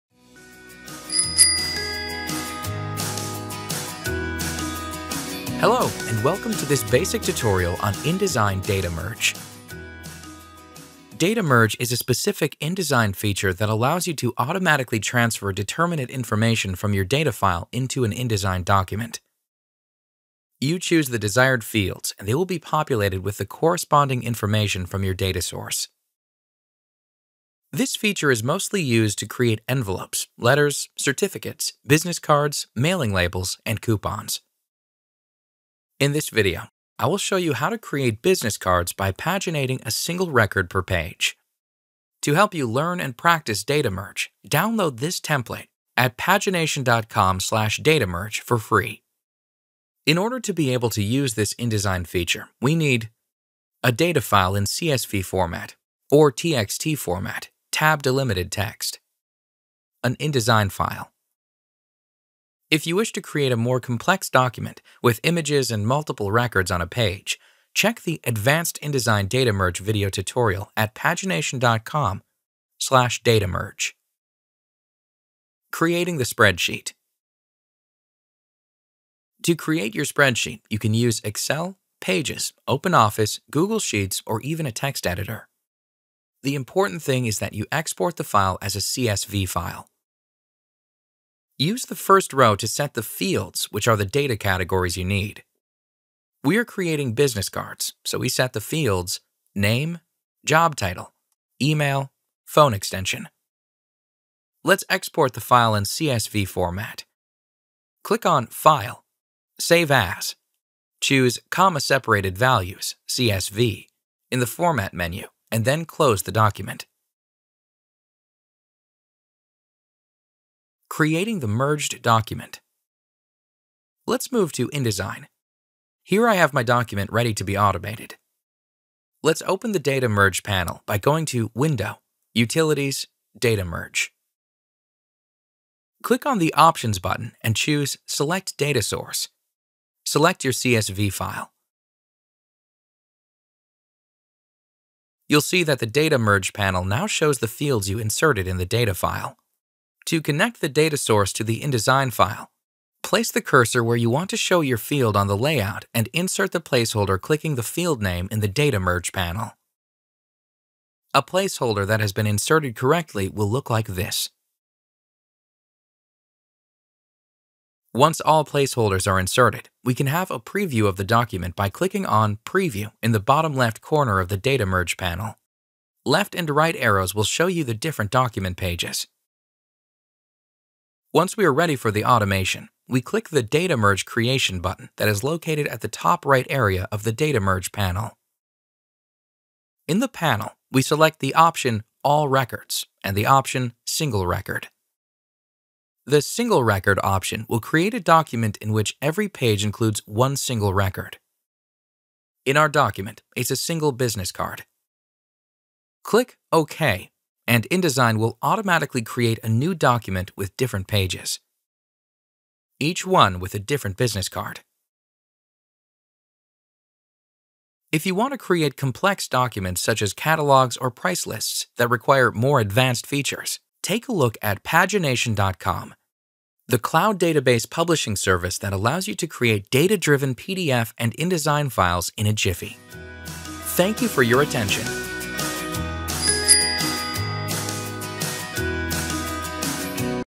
InDesign Data Merge - Basic Tutorial (Single record per page)
Most mentioned InDesign Data Merge Limitations:

- Cannot group products;
- Cannot use multiple product templates;
- Cannot insert a page break when the category changes

To overcome these limitations, try Pagination – The Turnkey Database Publishing Service that allows you to create data-driven PDF and InDesign files in a jiffy.